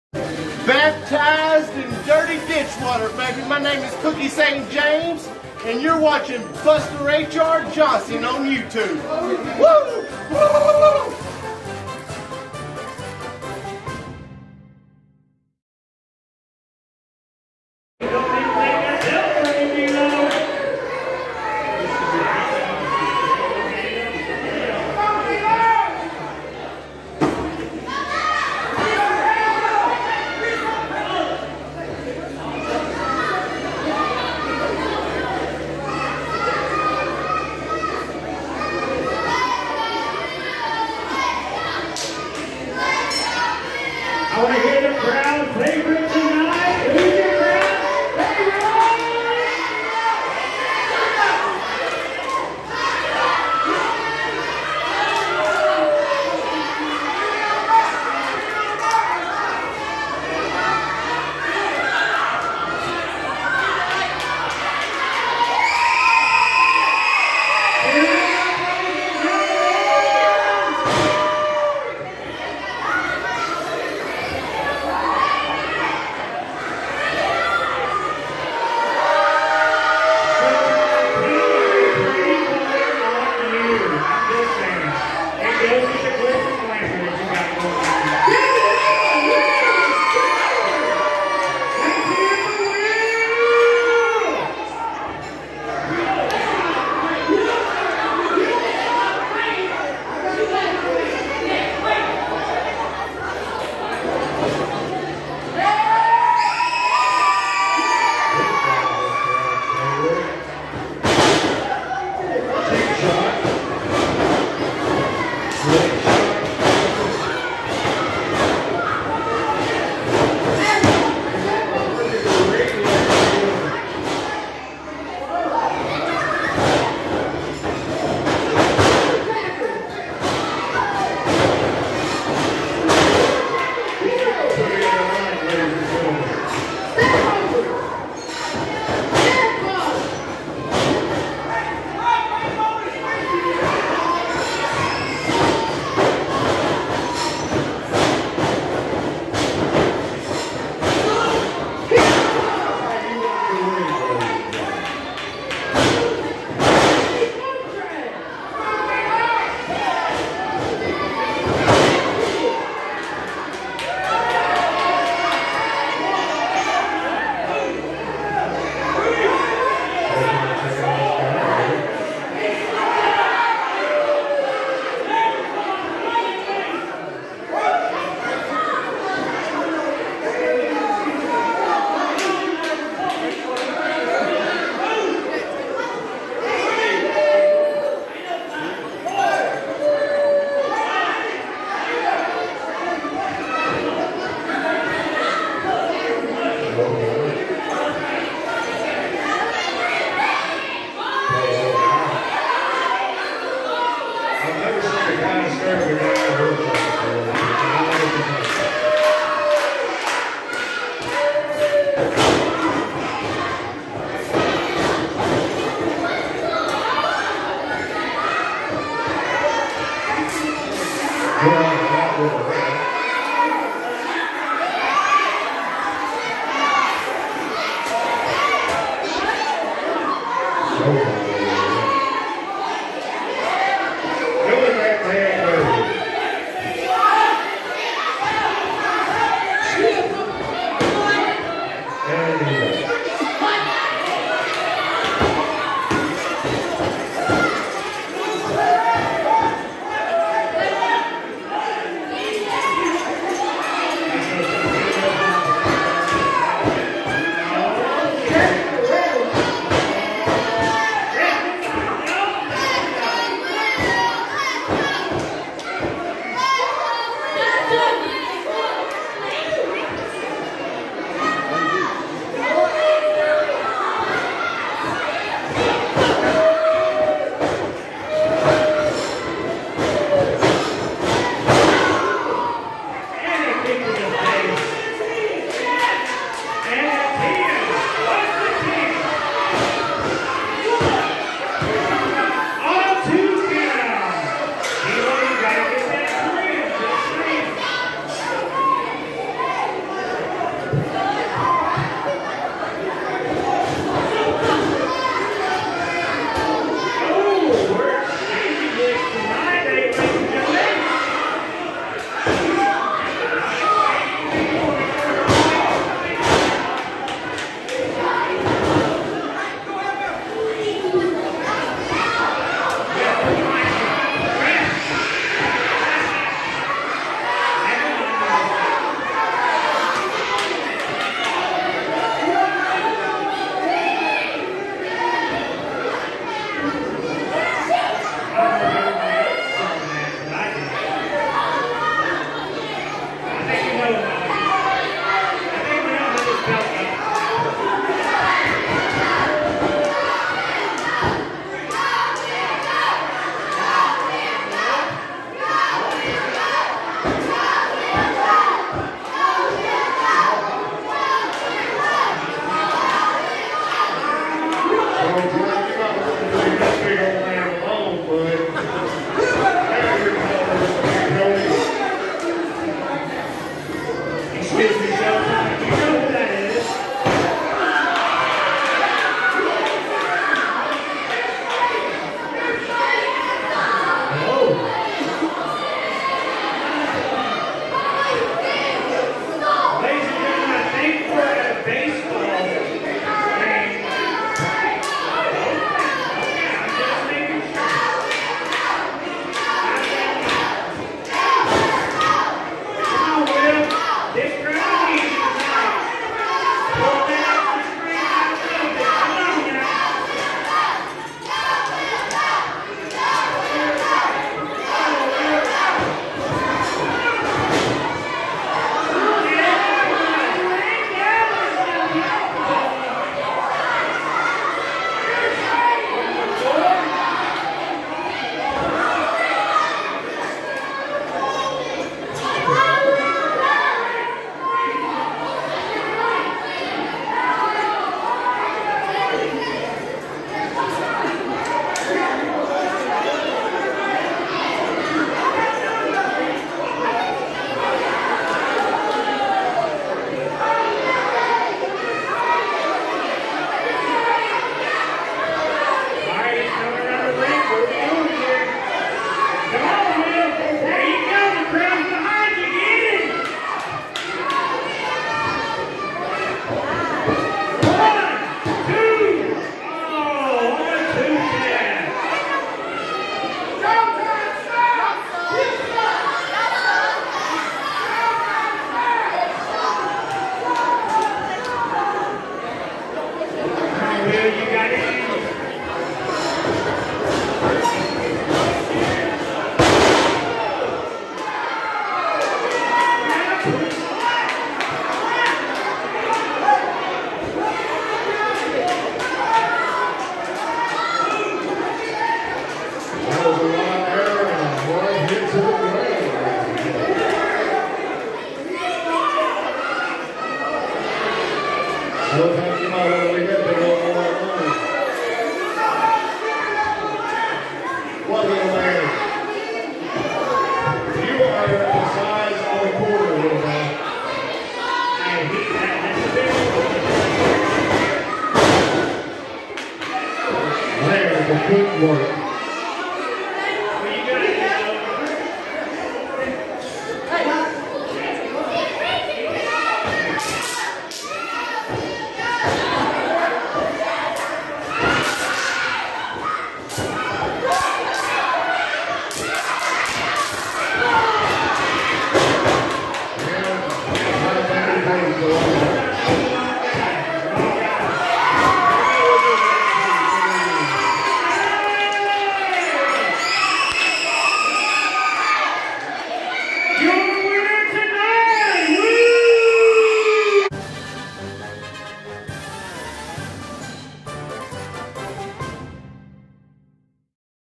WILL SIMS vs D LO W SHOWTIME (EIW DYER TN 4/1/2022)
THIS IS FROM EIW AT THE DYER TN. FIRE DEPT 4/1/2022. THIS SHOW WAS TO HELP RAISE MONEY FOR THE DYER FIRE DEPT.